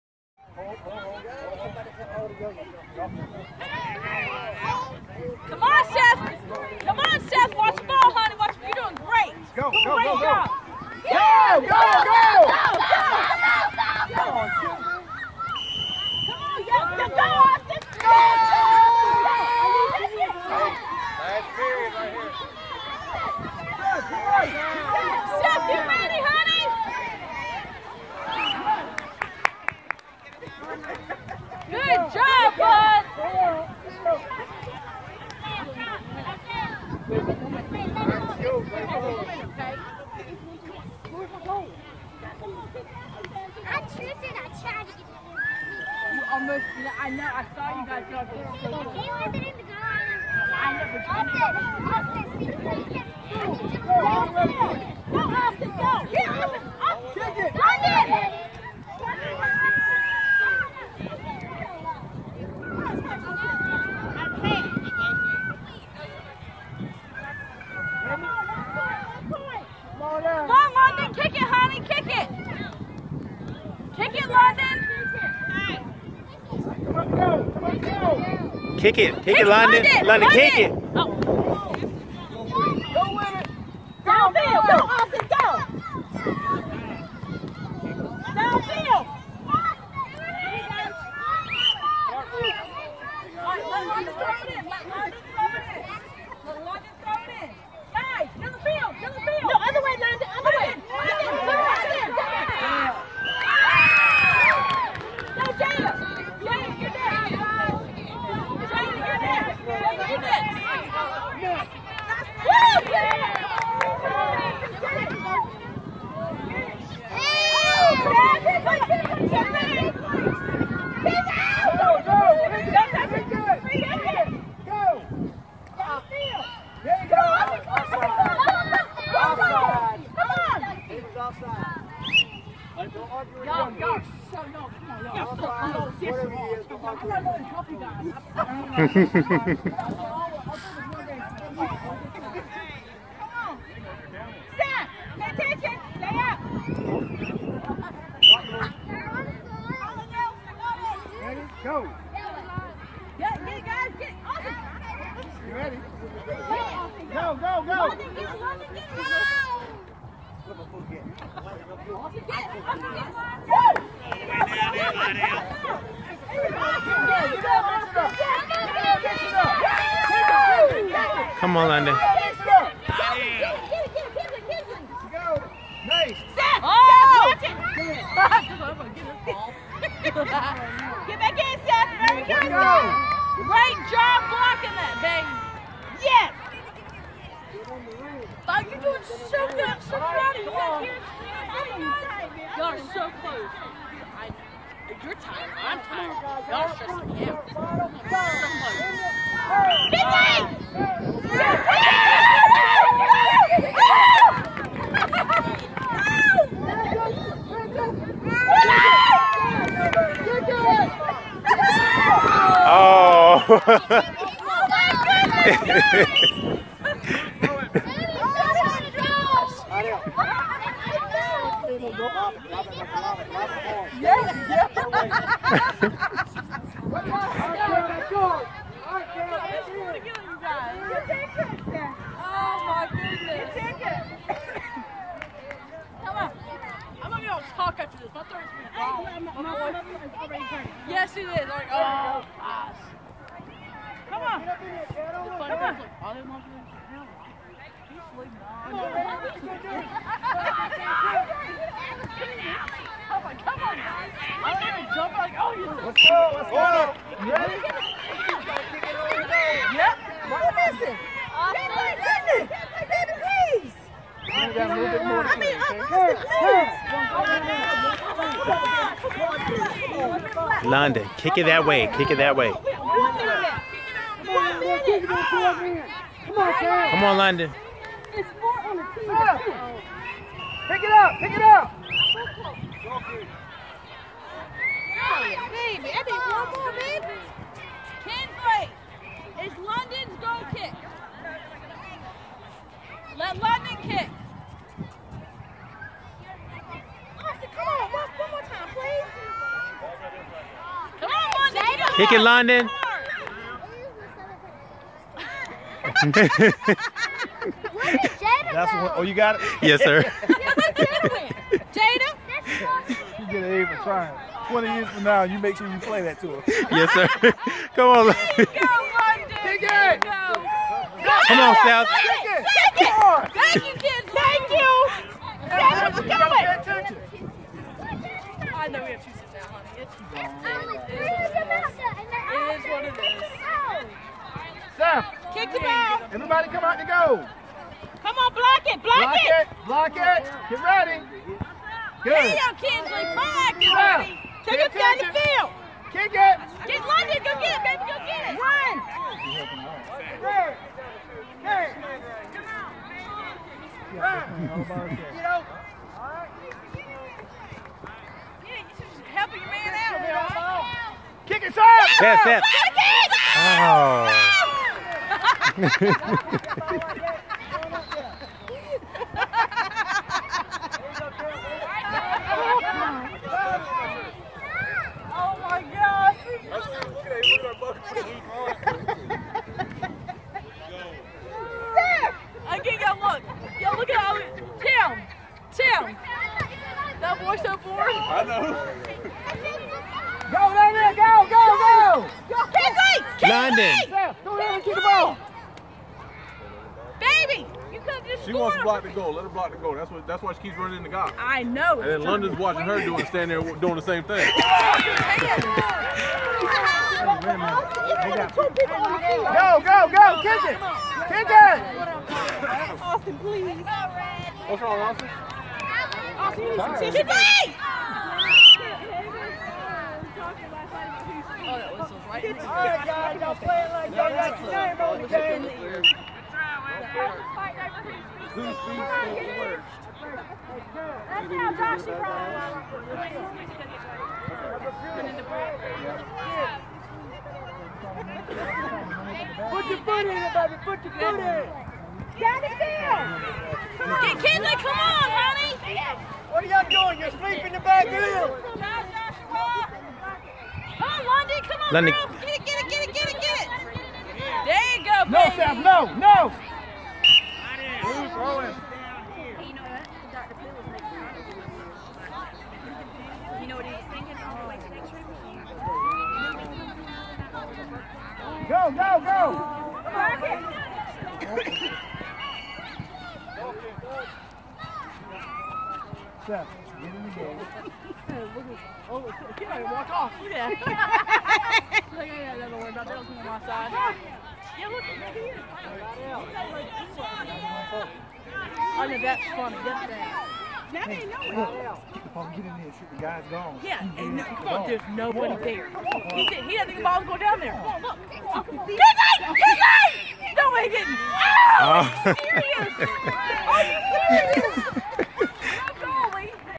Londyn's soccer game
At 5 mins into video, princess Londyn attempts to hit the soccer ball 😄😁